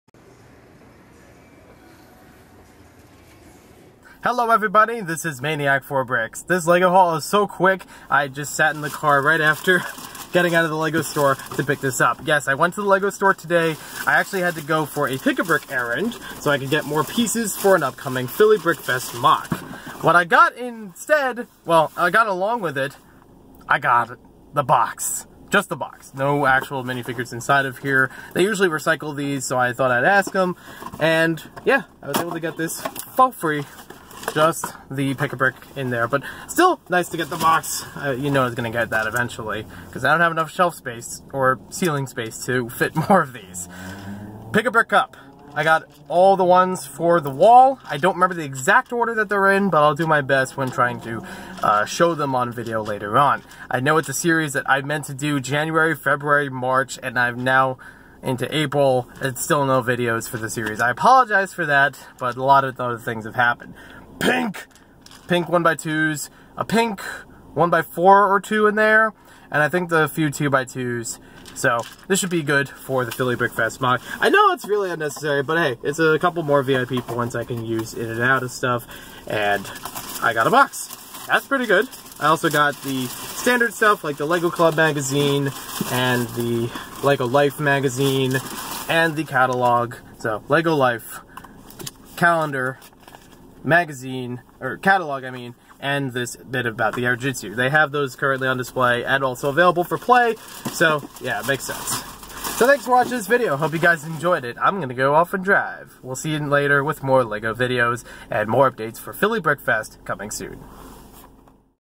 Philly Brickfest pickup from The LEGO Store!
Topping off a movie and hunting classic policemen with the LEGO Store near me!